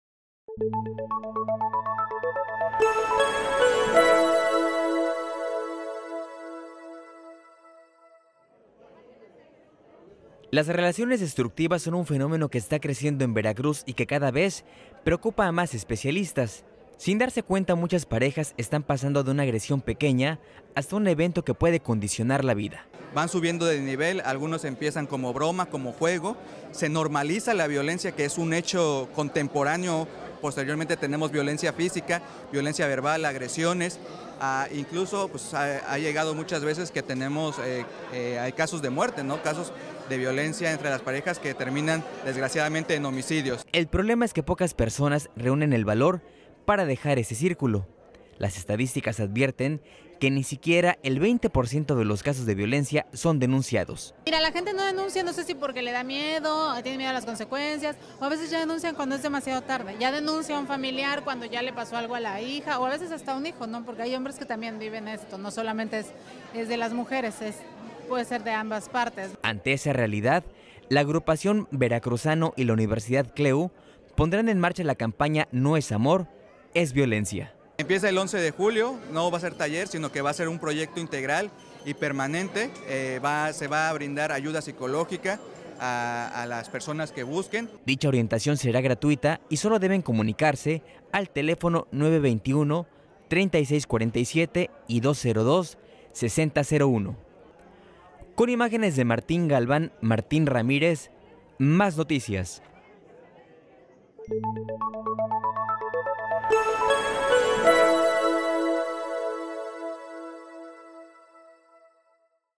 Aumentan la violencia en el noviazgo; inician campaña para prevenirla
studiantes de la Universidad CLEU e integrantes de la Asociación Civil Veracruzanos iniciarán la campaña "No es amor... Es violencia" para prevenir las agresiones de pareja desde el noviazgo.

Las relaciones destructivas son un fenómeno que está creciendo en Veracruz y que cada vez preocupa a más especialistas. Sin darse cuenta muchas parejas están pasando de una agresión pequeña a otras que ponen en riesgo la vida.